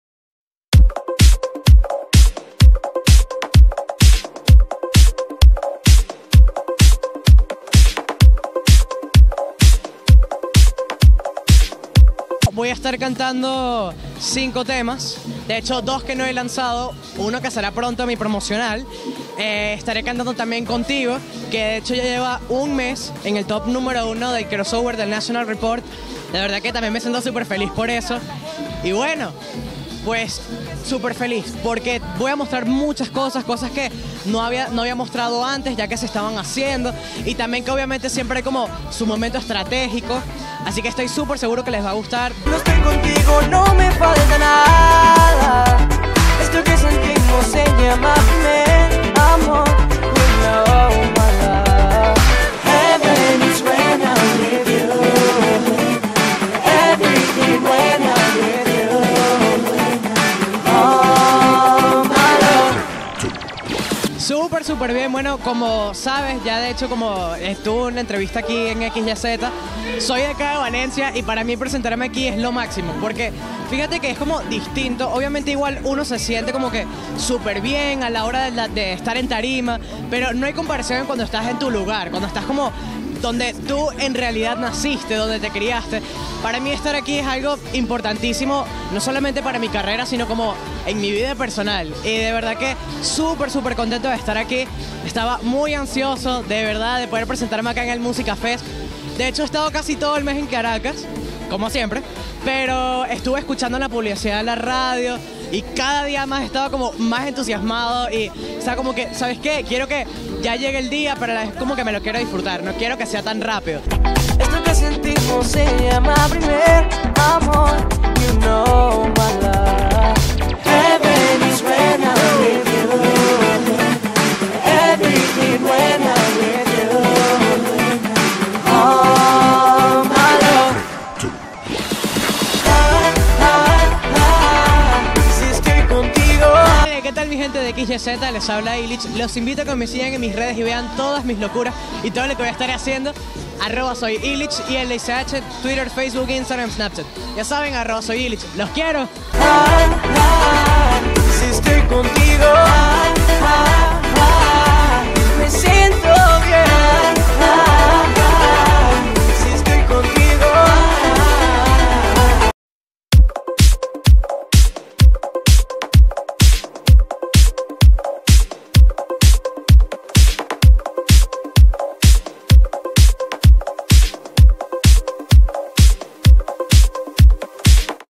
Ilich en el Musik Fest
Ilich también dijo presente en el Musik Fest, y el equipo de XYEAHZ logró conversar nuevamente con el y descubrir todas las sorpresas que esa noche le regaló al publico valenciano.